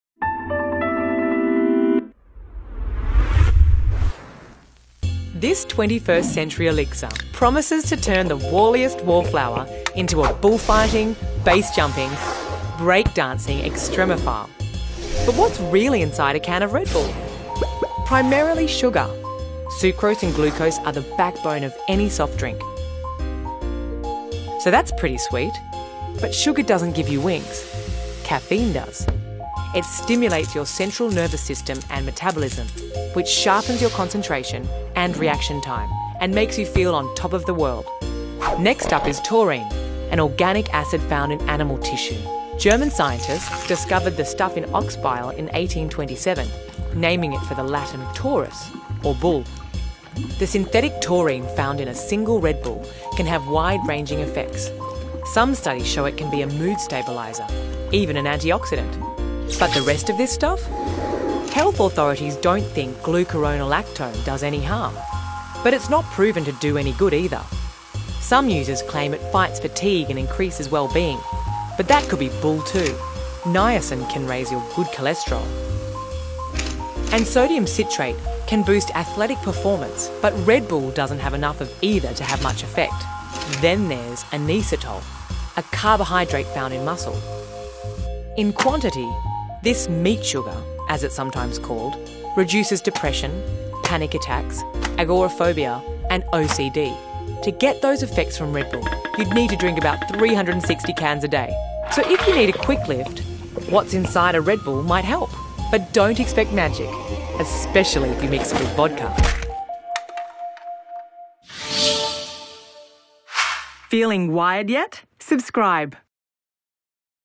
レッドブルの「元気が出る成分」の正体とは？| What's Inside | WIRED.jp
身の回りにある日用品の中に何が入っているかをCGアニメーションで解き明かす「What's Inside」シリーズ。日用品を解剖してみると、そこには思いもしなかった成分や、意外にハイテクな技術が注ぎ込まれていることに気付かされます。